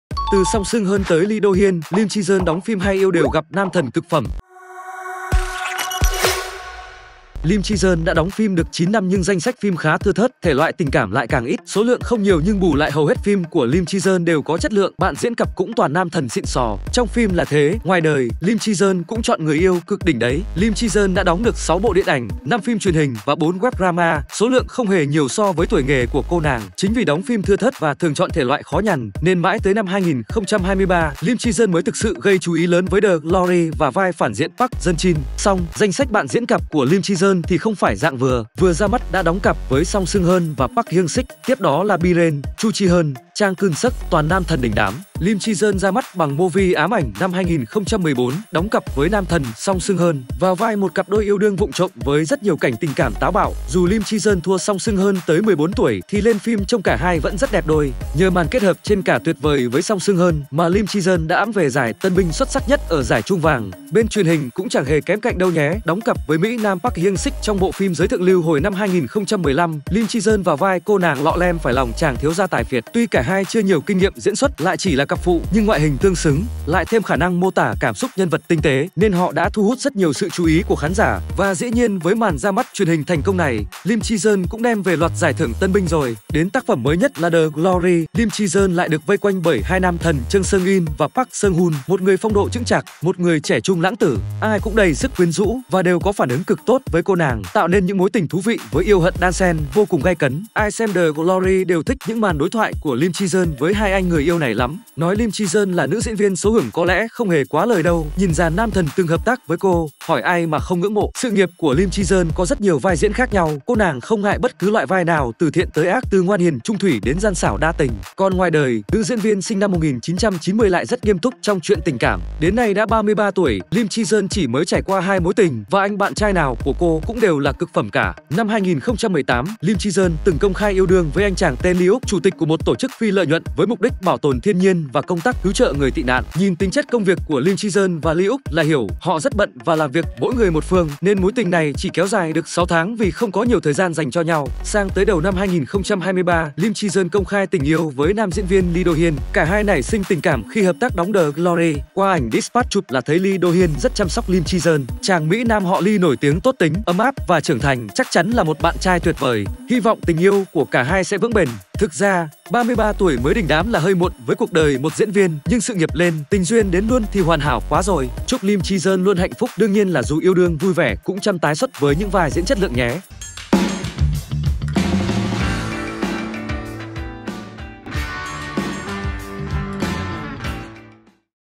Từ Song Seung Hun tới Lee Do Hyun, Lim Ji Yeon đóng phim hay yêu đều gặp nam thần cực phẩm
Lim Ji Yeon đã đóng phim được 9 năm nhưng danh sách phim khá thưa thớt, thể loại tình cảm lại càng ít. Số lượng không nhiều nhưng bù lại hầu hết phim của Lim Ji Yeon đều có chất lượng, bạn diễn cặp cũng toàn nam thần xịn sò. Trong phim là thế, ngoài đời, Lim Ji Yeon cũng chọn người yêu cực đỉnh đấy.

☞ Bản quyền thuộc về Trạm Tin Sao Hàn.